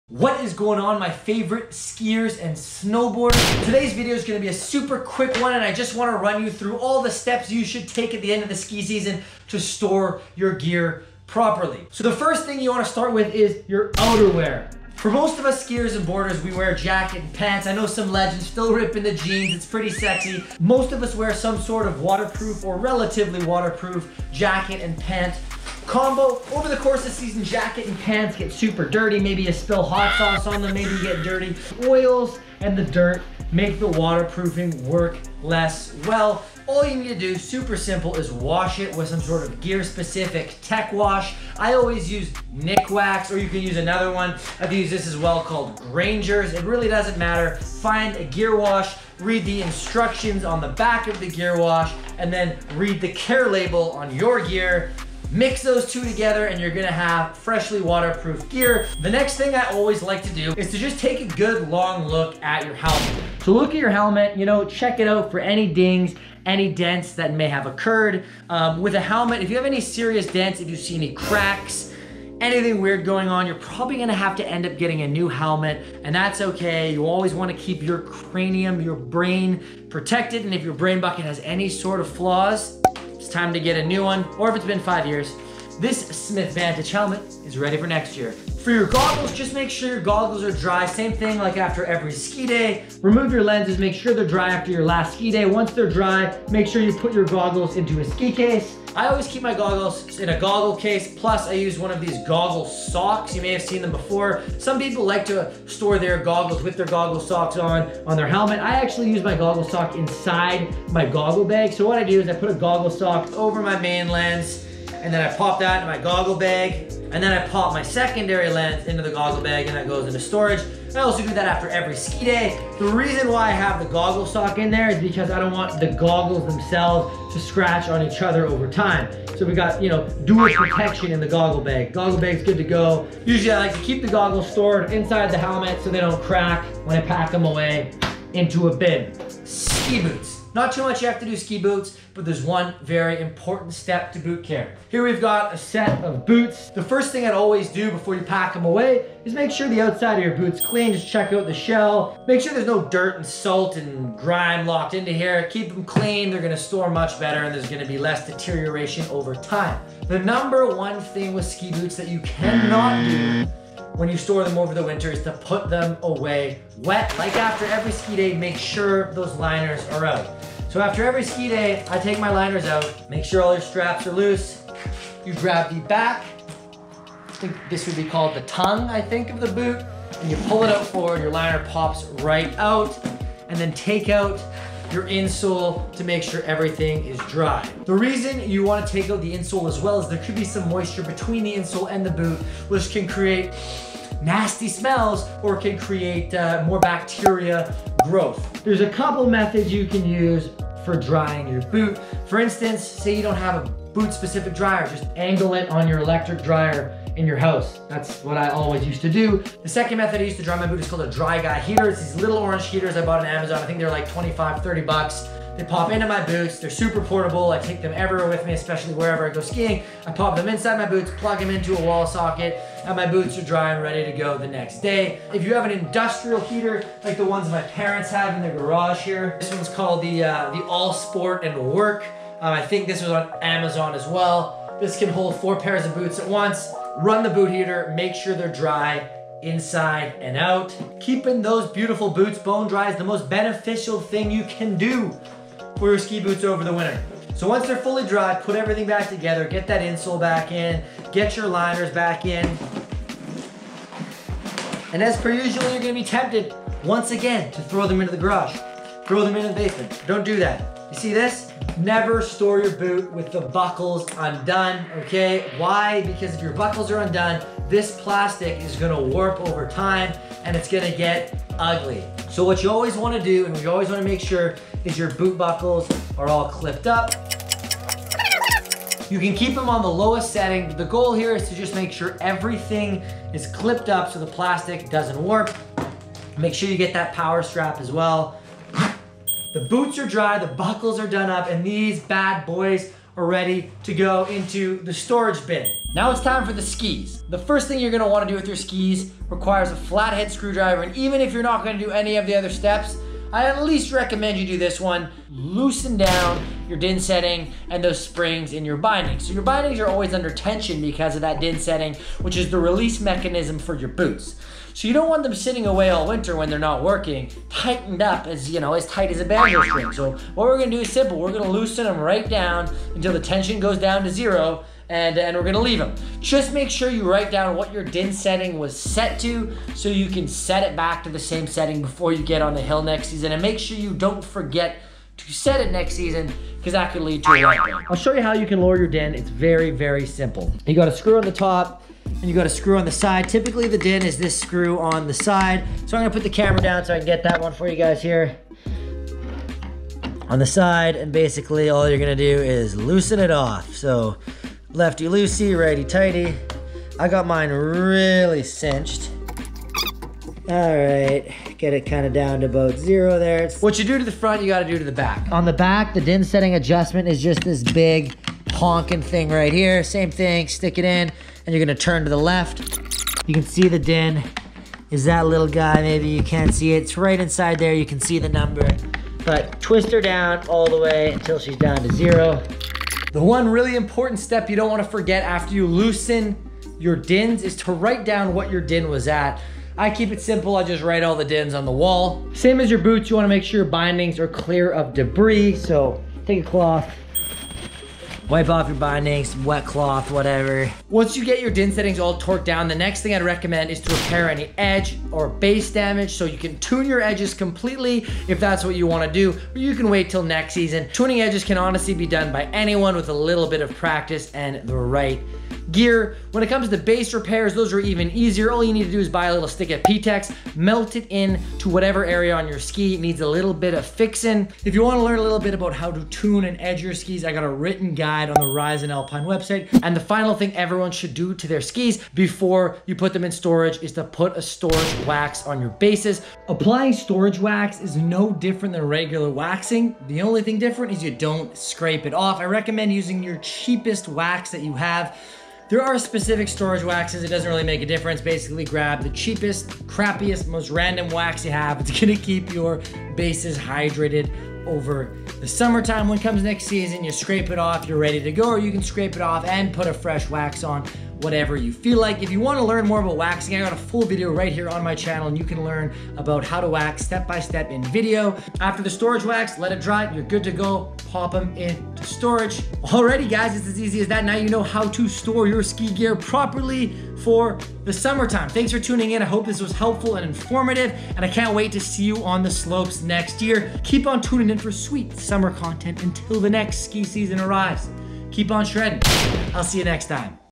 STOP Storing Your Ski Gear WRONG…. DO THIS INSTEAD!!!
Here are the 7 most important things you should be doing to properly store your ski gear in the offseason.

Proper maintenance and storage of your ski gear will extend the life of your equipment and allow you to perform at your best on opening day.

Gear I Use In This Video:
NikWax Tech Wash
Dry Guy Boot Dryer Portable
Boot Dryer Large
Storage Wax
Waxing Iron

00:00 – Intro
00:15 – Outerwear
01:04 – Helmet
01:29 – Goggles
02:22 – Ski Boots
05:07 – Lower DIN Setting
06:45 – Tune Edges & Repair Base Damage
07:15 – Storage Wax